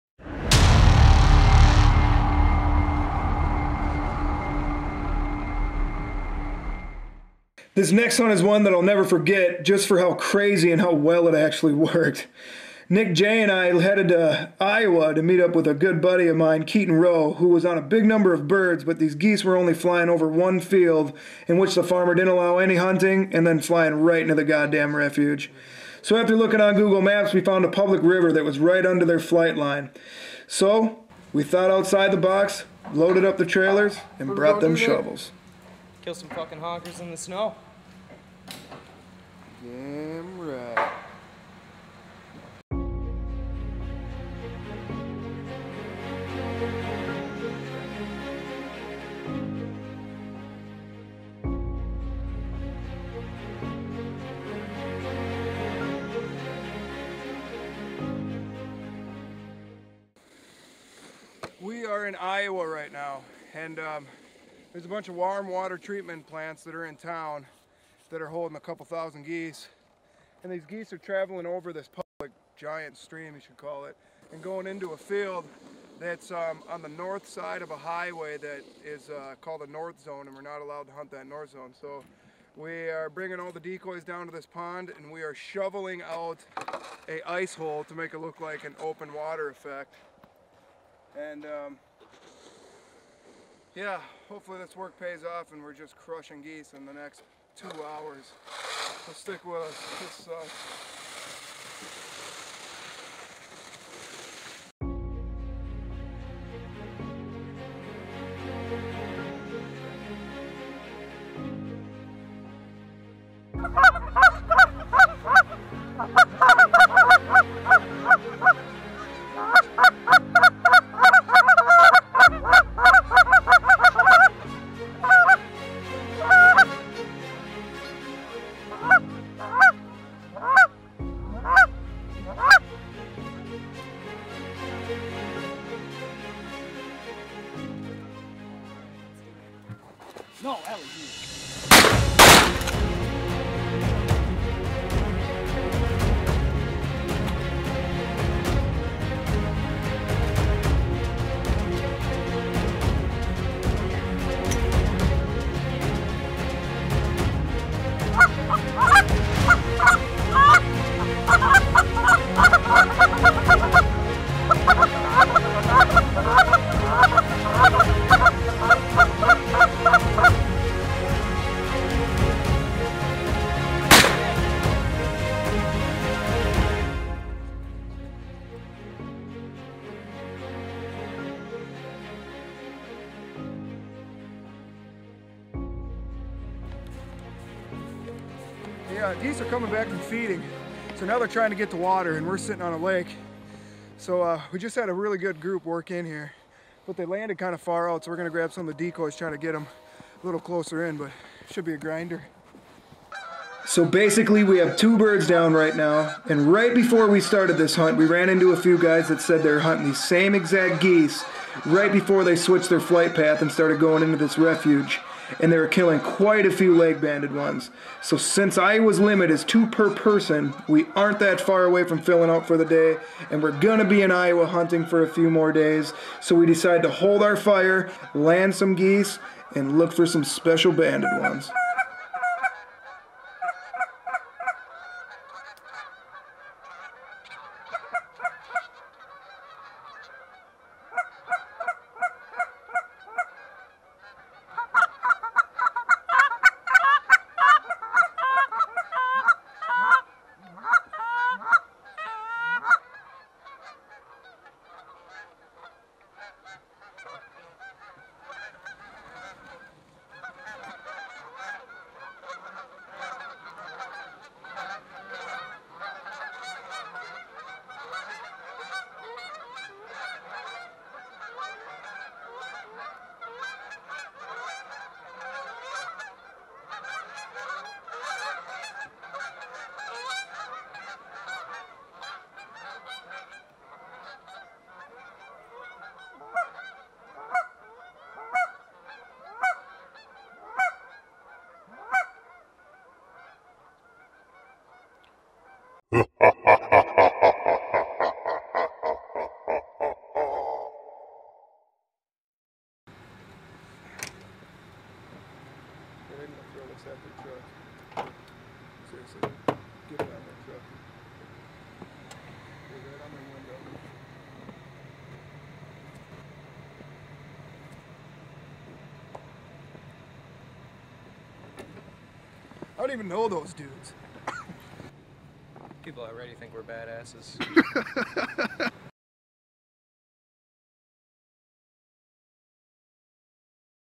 Waterfowl Hunting - MOST EPIC & UNIQUE GOOSE HUNT on Youtube!
Fowl Family's Joe Heintz and Nick Johnson are at it again with yet another crazy UNIQUE Canada goose hunt! this time we are in Iowa chasing the last few geese to close out the Iowa season . birds are few far and between at this point gotta do your major scouting miles to find the last few GIANTS. local power plant has some warm water holding ponds and the geese are STACKED only problem these birds leave city limits fly one mile and dumb into a field thats in refuge with only one field to get under them we open our plot books and cell phones and got a big fat NO go from the land owner on the only shot they had. when every one else woulda packed up and headed home we new there had to be a way. a little public fish and hunt river these geese were corning over was just what they wanted to see on Google Maps. what might sound crazy and had every one saying "no way" but we knew with a big spread and aggressive calling and a tempting looking hole ;) we would get them geese to finish . finish hard in our ice hole. ENJOY